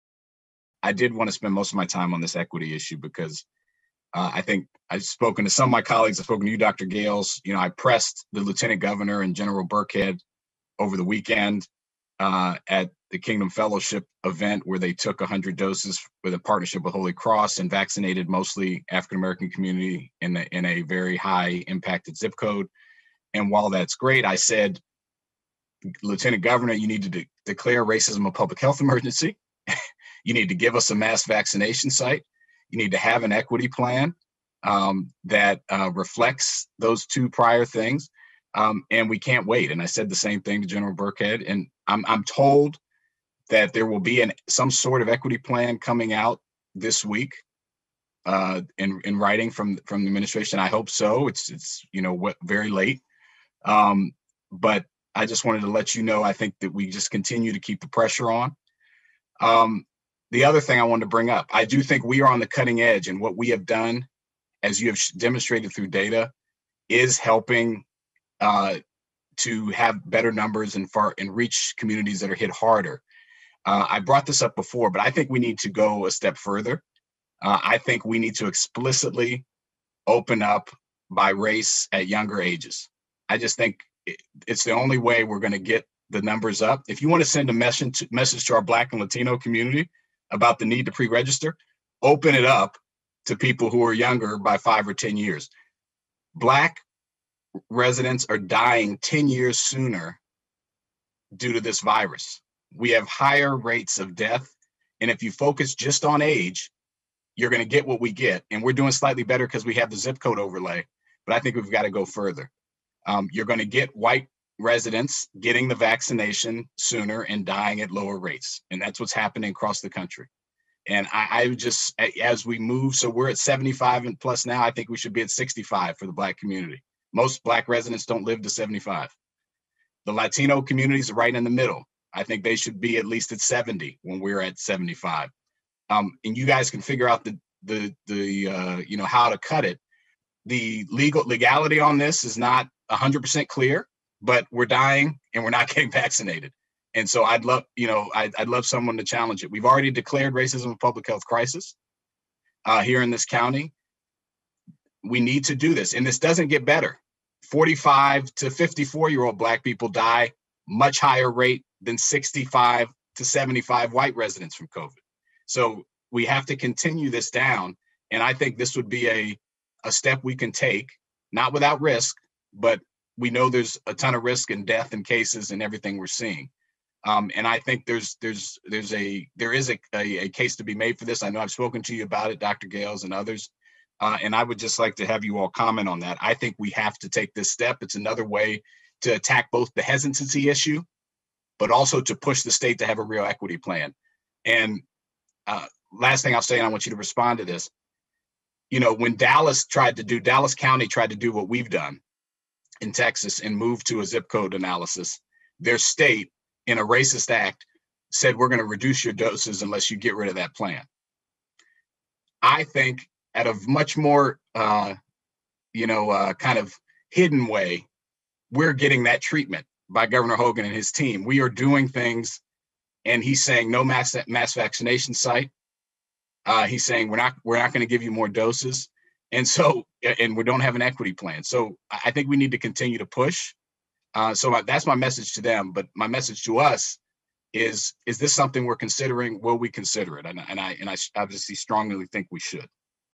Will Jawando Vaccination Equity
Montgomery County Councilmember Will Jawando discussed the importance of racial equity in the distribution of the county's Cover-19 vaccination program.